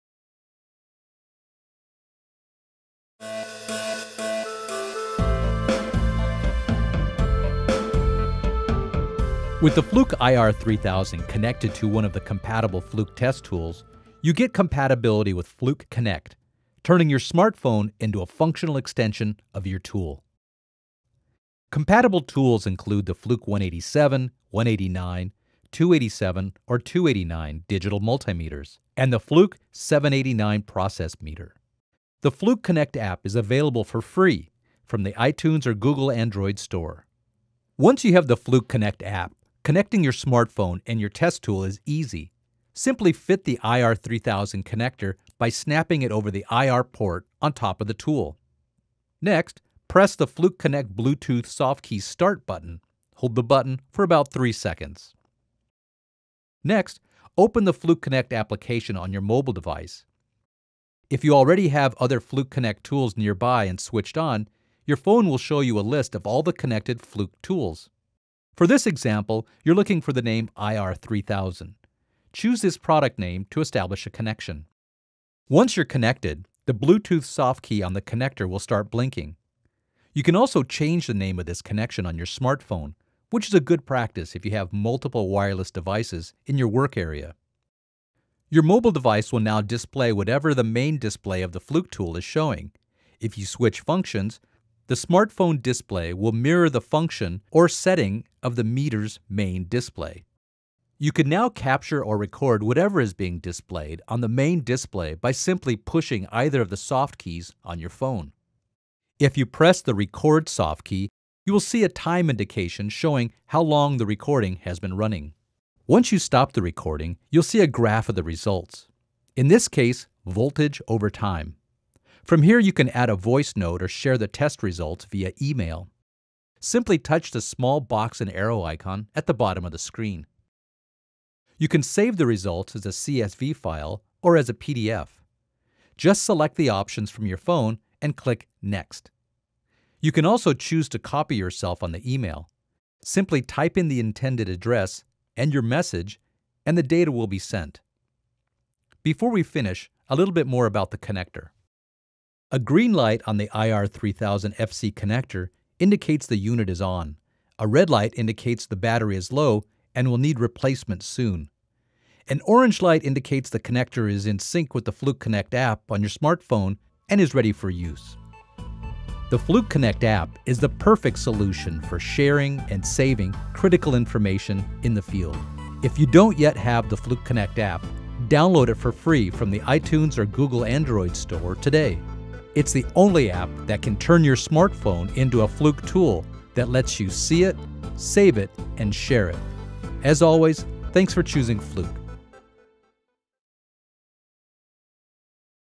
Using Fluke ir3000 FC Connector and the Fluke 289 Multimeter
This video will demonstrate how to connect the Fluke ir3000 FC Connector with Fluke 289 and 287 True-RMS Multimeters. Basic operation is shown, along with how measurement results from the 280 Series DMM are transmitted to the user’s smartphone.
Learn more about the ir3000 FC Connector.